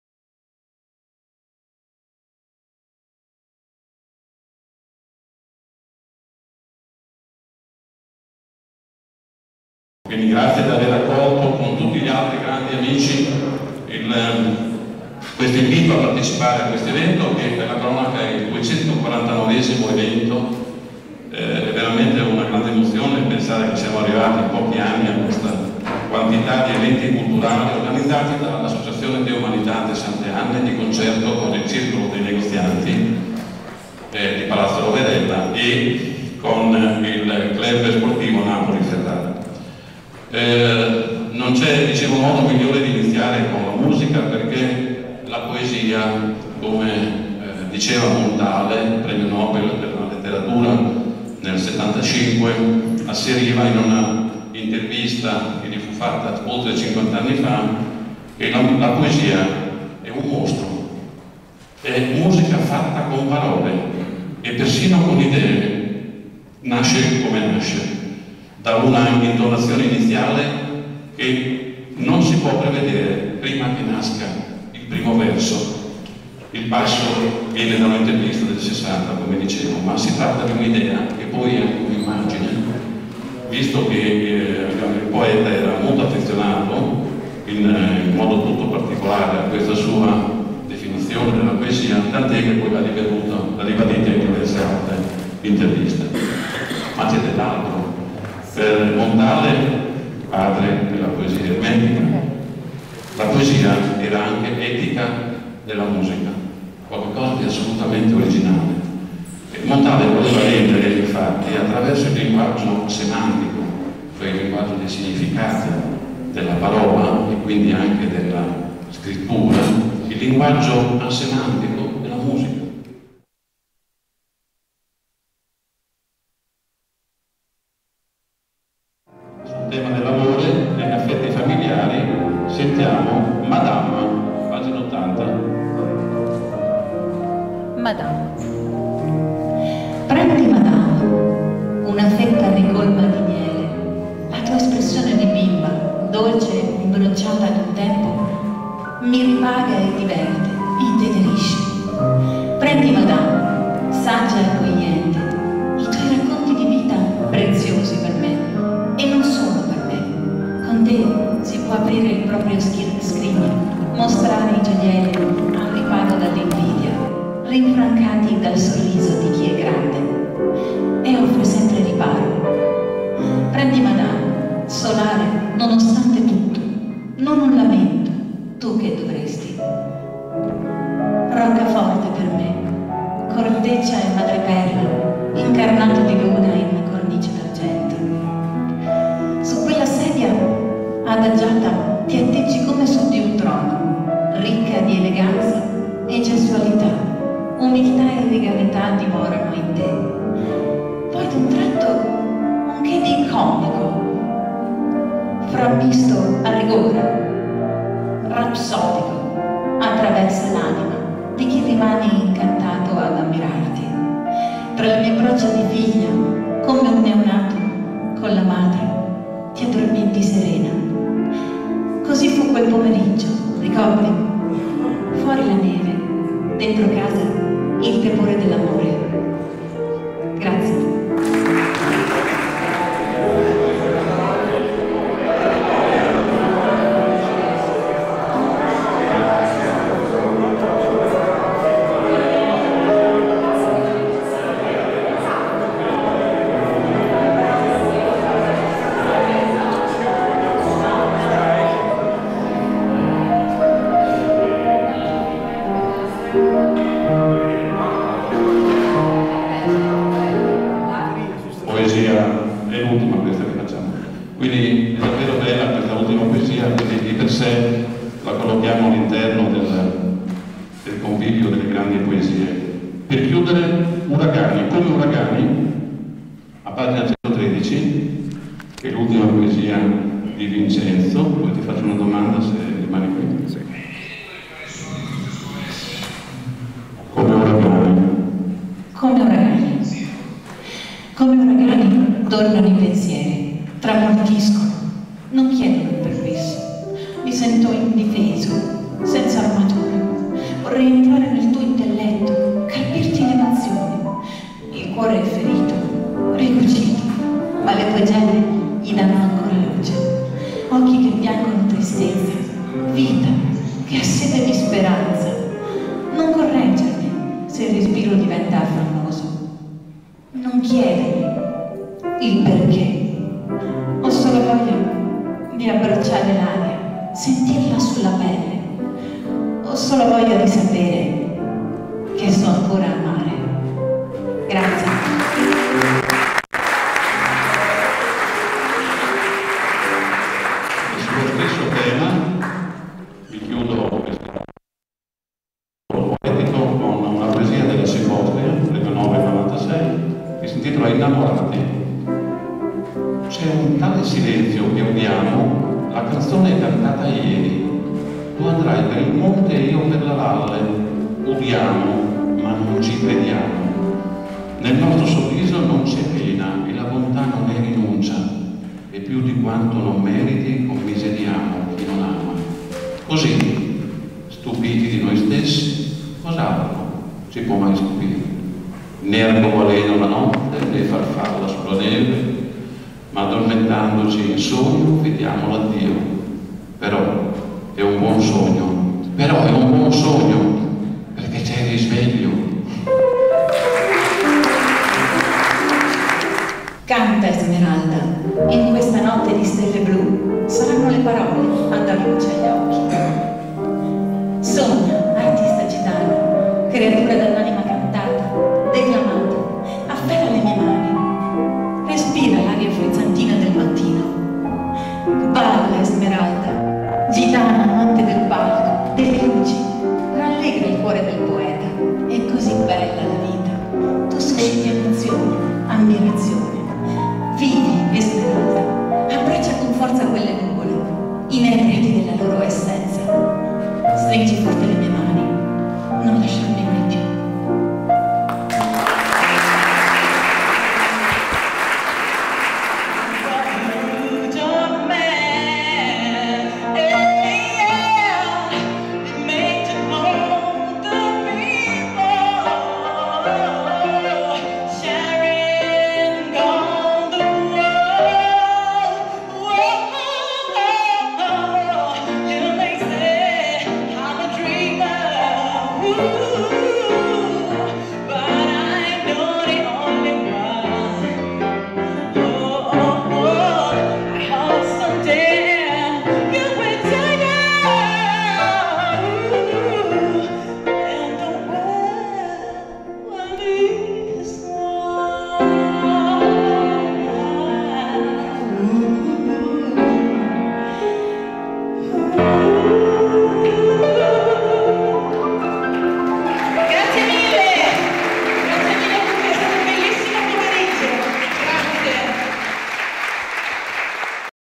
Mix video - Presentazione libro "Poeti al Trivio - Dissonanze"
Mix audio alla presentazione libro "Poeti al Trivio - Dissonanze" dei tre autori. Nicola Corrado - Rita Consiglio e Vincenzo Russo. Palazzo Roverella a Ferrara